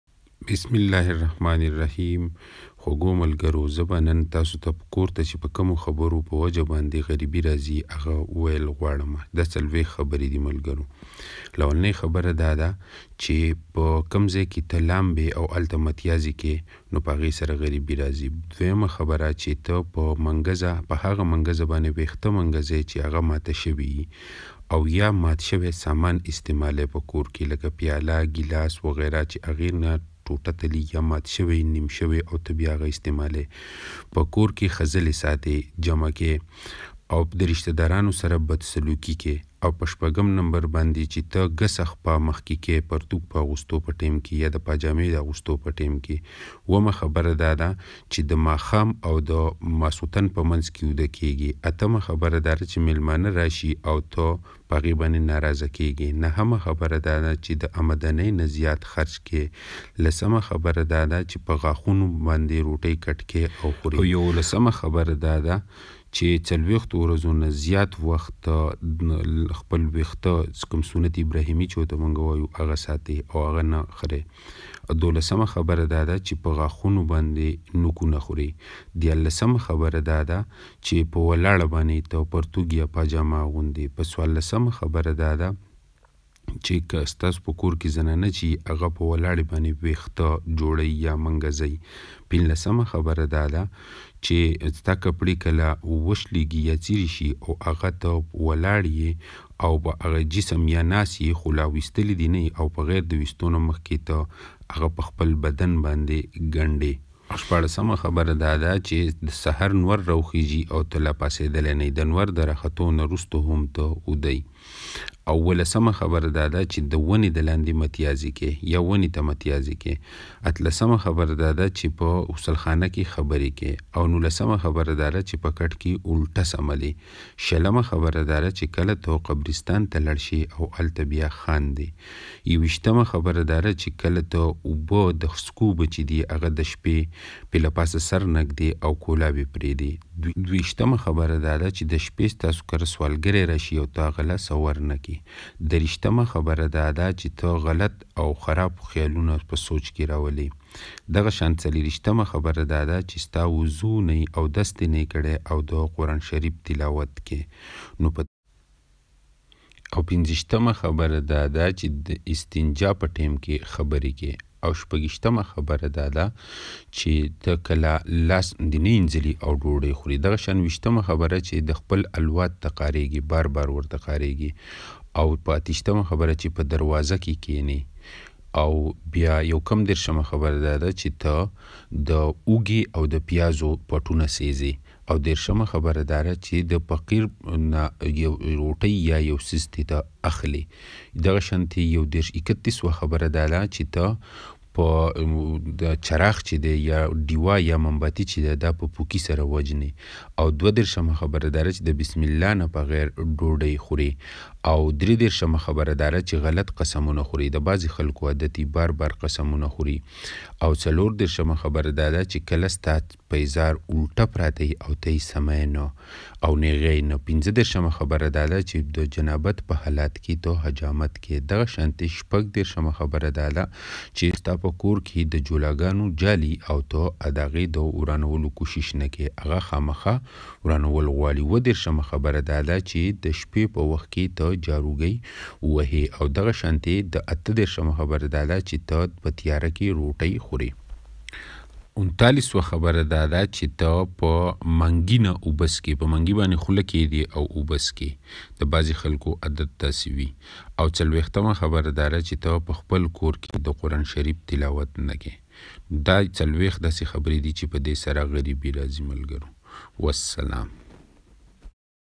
Rules to become rich | For Poor people
This video show you why some home became poor and poorer. Follow the rules and became rich In Sha Allah.